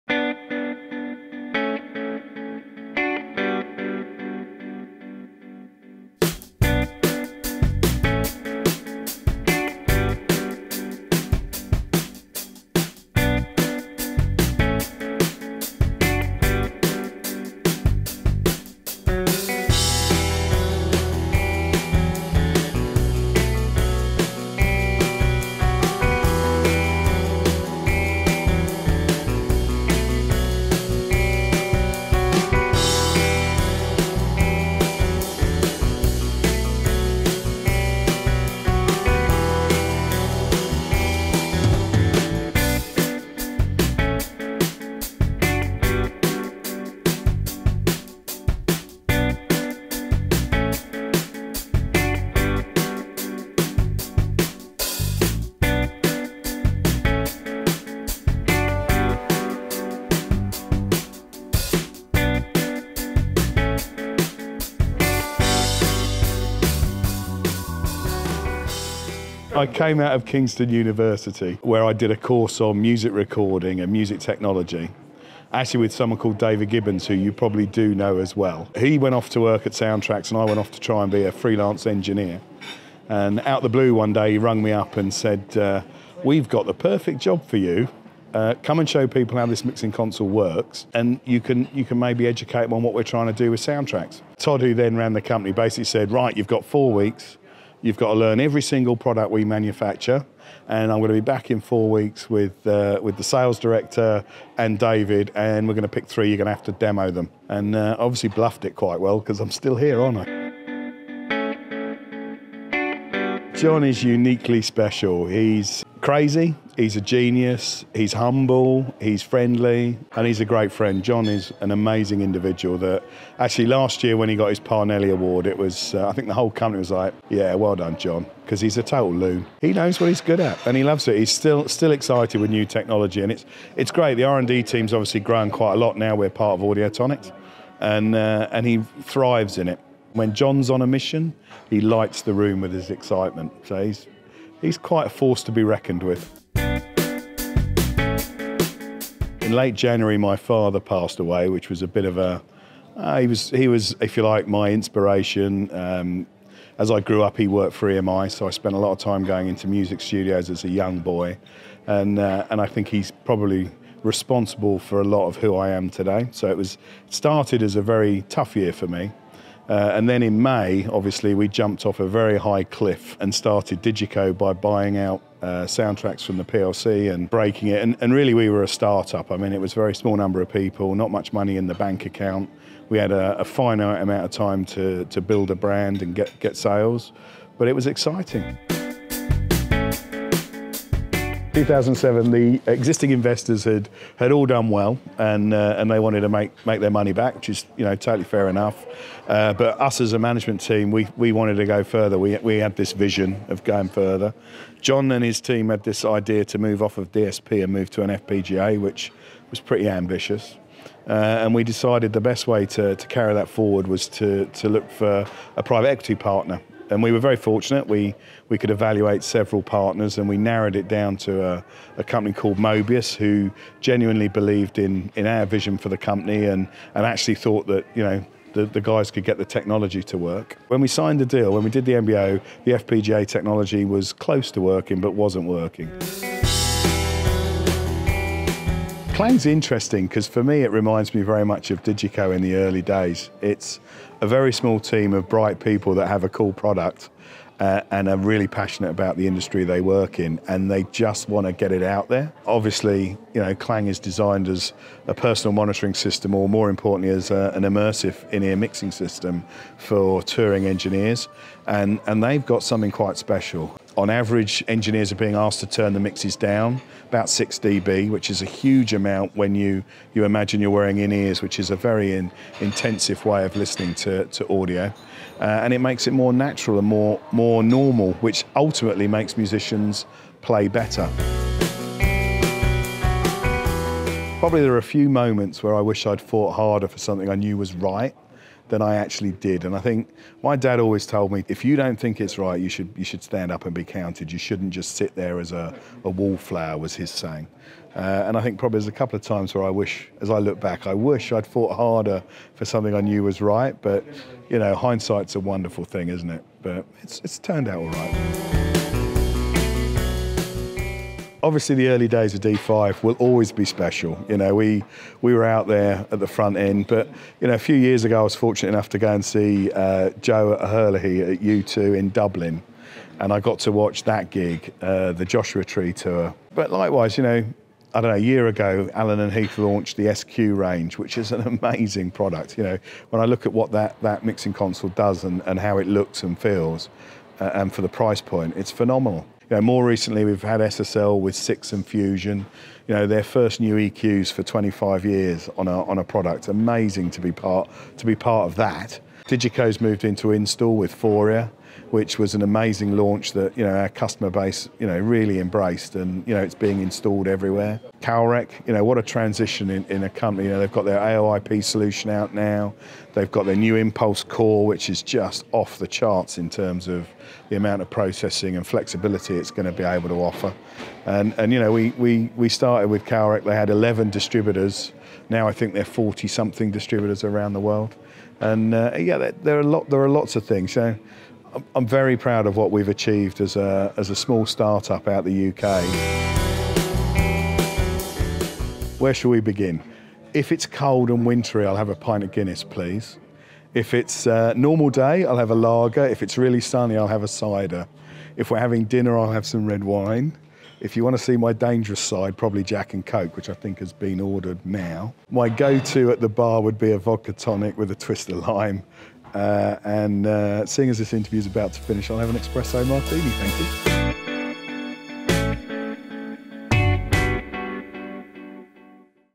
Industry Drivers: DiGiCo
Industry Drivers returns with Audiotonix CEO James Gordon in the driver's seat. He discusses his start in the industry, the genius of John Stadius, DiGiCo's founding in 2002, his proudest moment & his favourite drink.